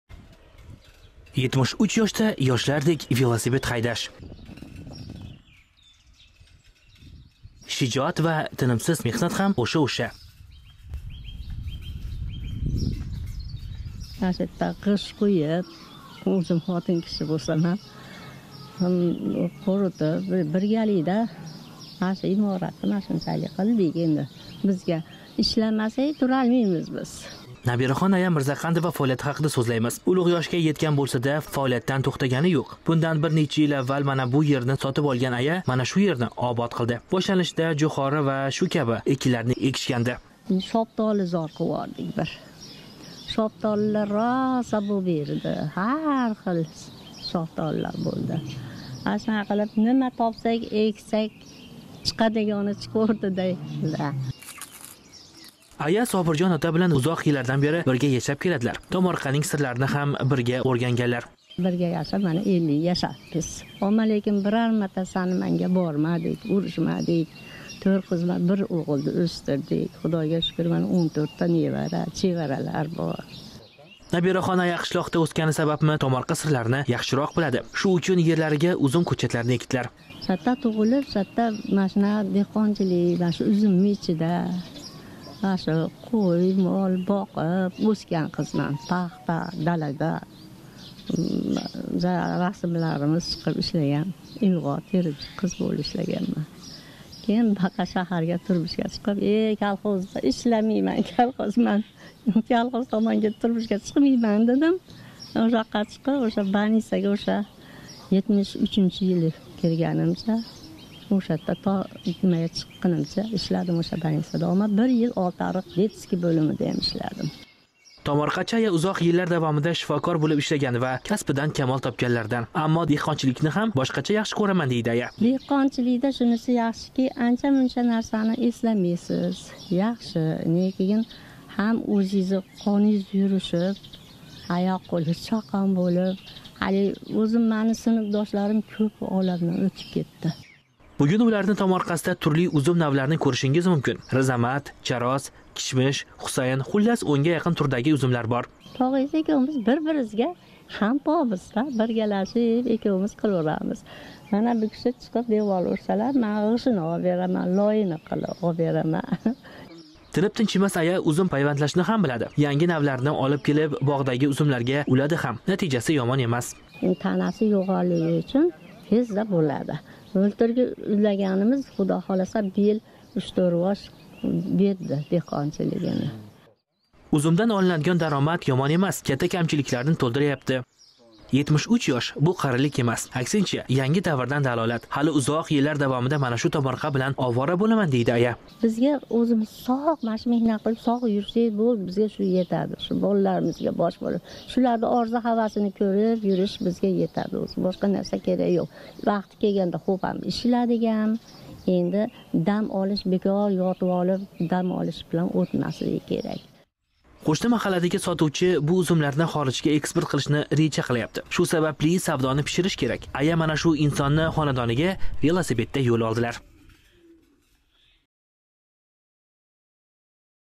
عهده‌بوران اۉزبېک آنه‌خانی
عهده‌بوران اۉزبېک آنه‌خانی همه‌نی حیران قالدیرگن. قنده‌ی دېیسیزمی؟ گزارشنی تماشا قیلینگ